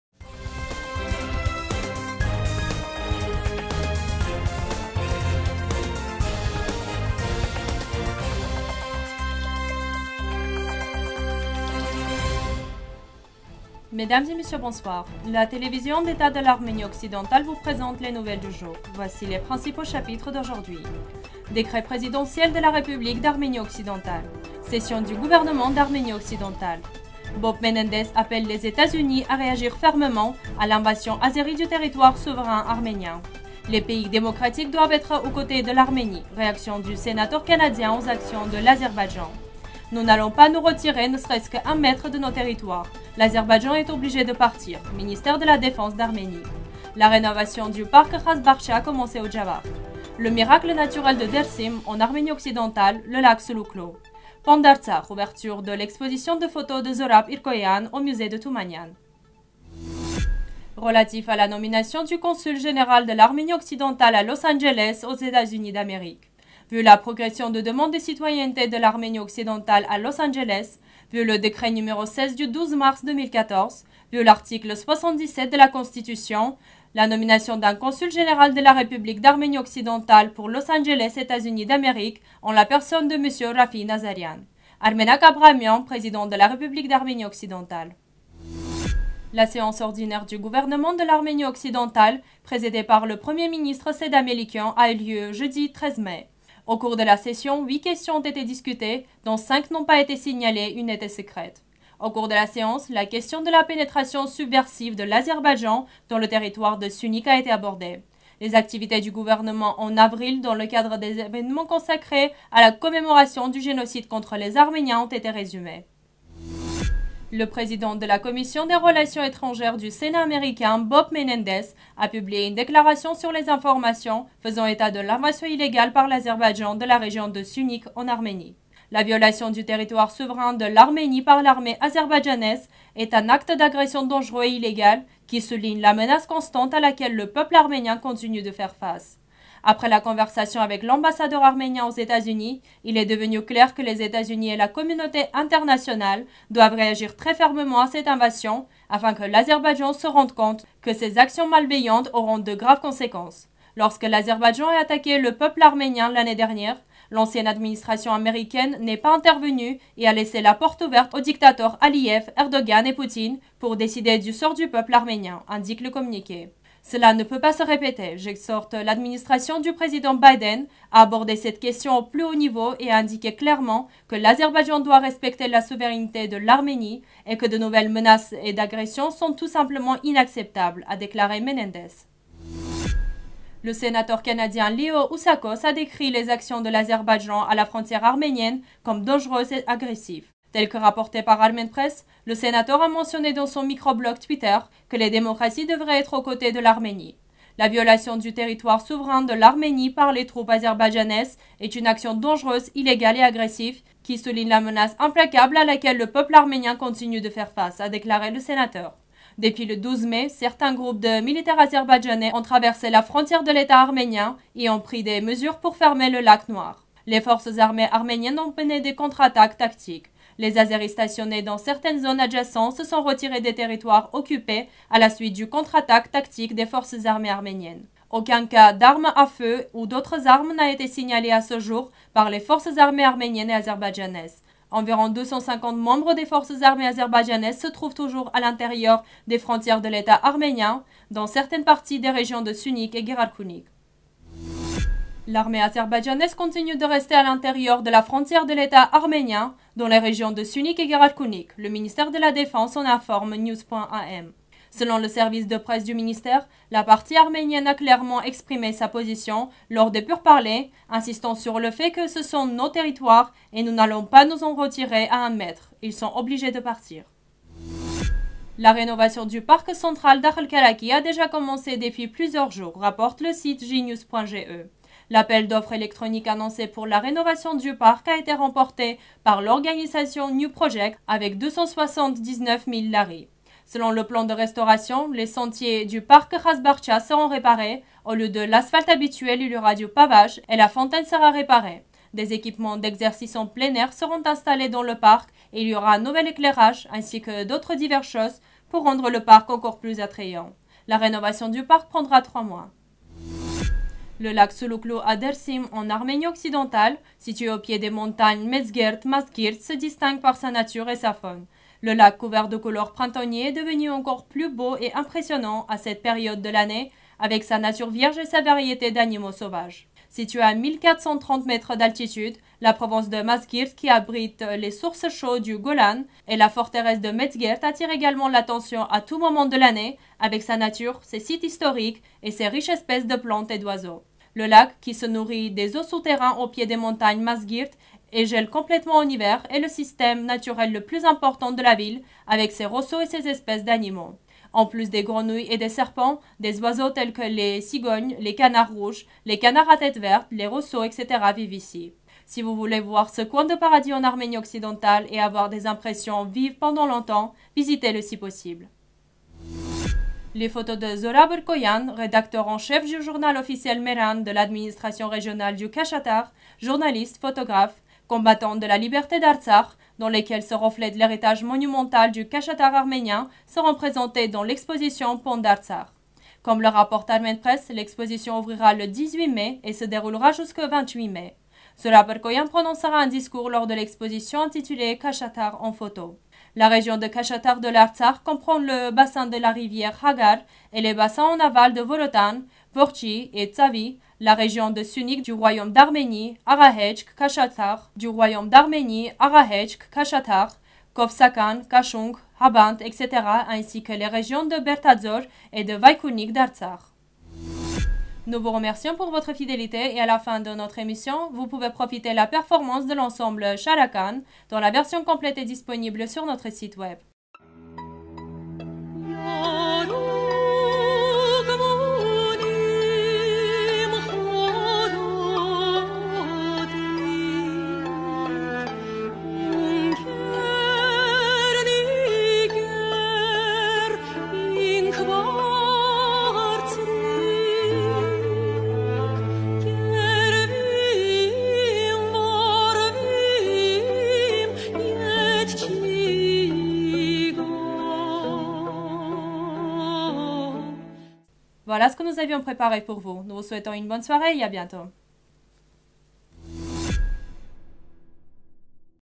Le miracle naturel de Dersim en Arménie Occidentale - Lac Suluklu․Nouvelles 14.05.2021
1. Décret Présidentiel
2. Session du Gouvernement d’Arménie Occidentale
3. Bob Menendez appelle les États-Unis à réagir fermement à l'invasion azérie du territoire souverain arménien
4. Les pays démocratiques doivent être aux côtés de l'Arménie. Réaction du sénateur canadien aux actions de l'Azerbaïdjan
5. Nous n'allons pas nous retirer ne serait-ce qu'un mètre de nos territoires, l'Azerbaïdjan est obligé de partir. Ministère de la Défense d’Arménie
6. La rénovation du parc Khas Bakhcha a commencé au Djavakhk
7. Le miracle naturel de Dersim en Arménie Occidentale - Lac Suluklu
8. Ponts d'Artsakh. Ouverture de l'exposition de photos de Zohrab Irkoyan au musée de Toumanian